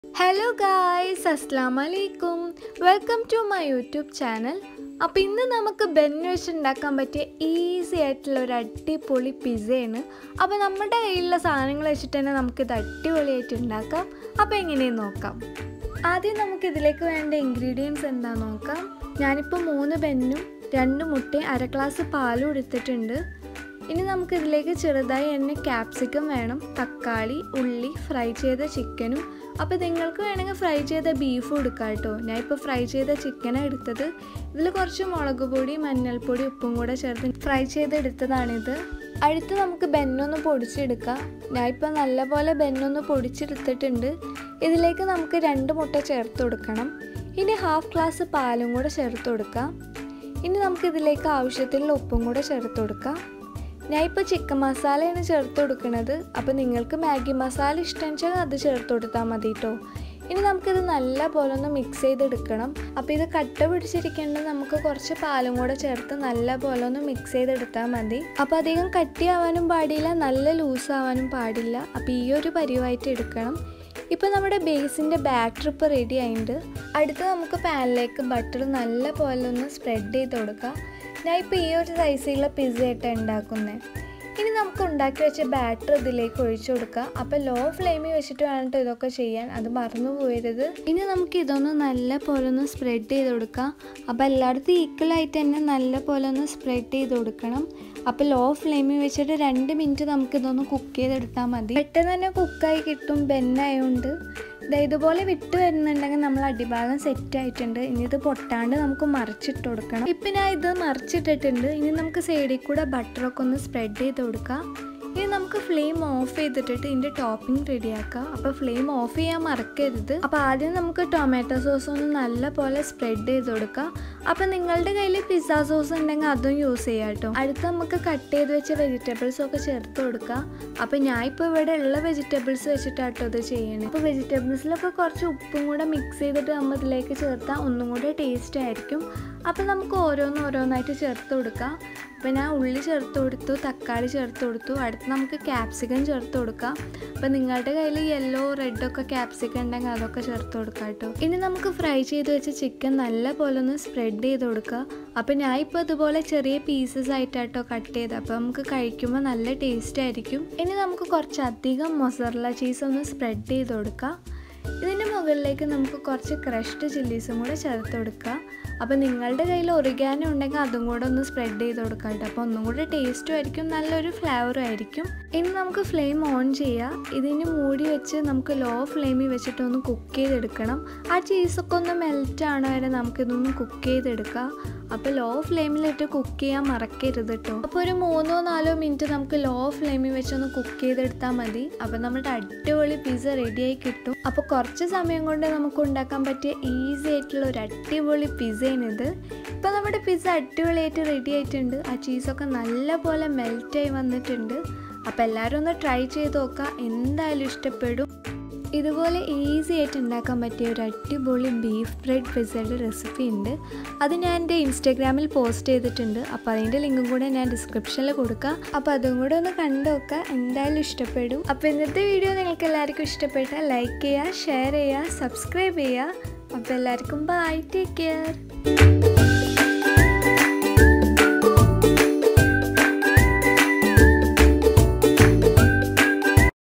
BUN PIZZA making 😋😋😋|| VEGETABLE and CHICKEN topping | Easy and tastyRecipe 😋😋👌🏻
Beef bread pizza😋😋👇🏻👇🏻👇🏻

*INGREDIENTS*
•Bun-3
•egg-2
•Milk-half glass
•Capsicum
•Tomato
•Onion
•Fried chicken
•Butter
•Tomato sauce
••mozzarella cheese
•chilly flakes